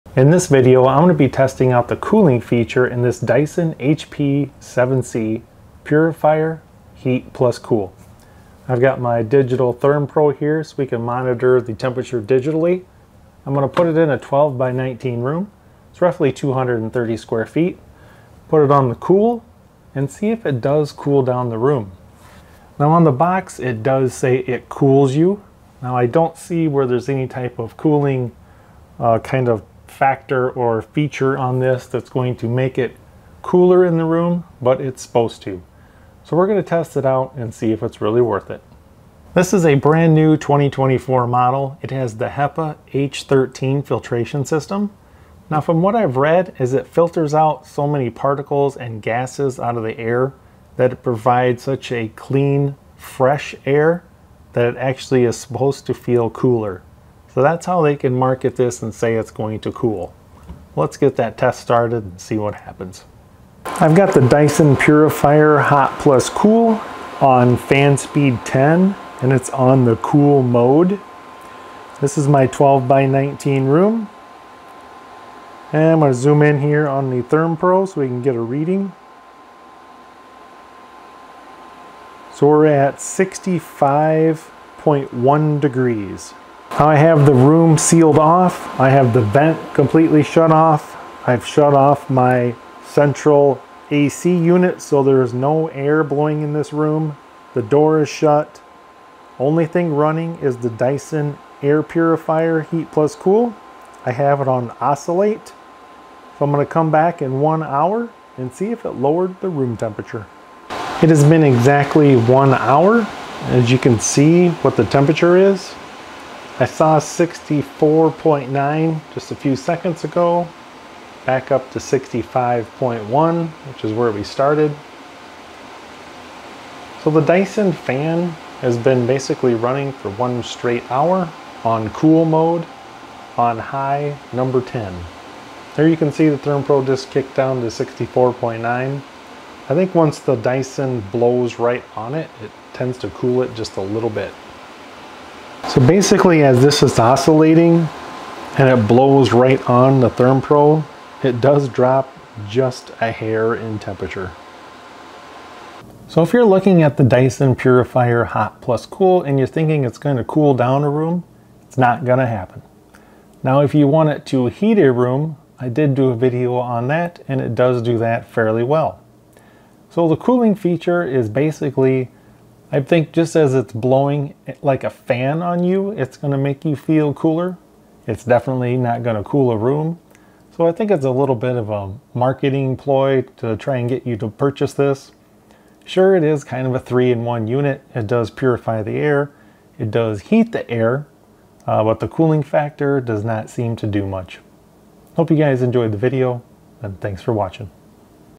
Will it COOL a Room? Dyson Purifier Hot + Cool
Will the dyson actually cool down the temperature of a room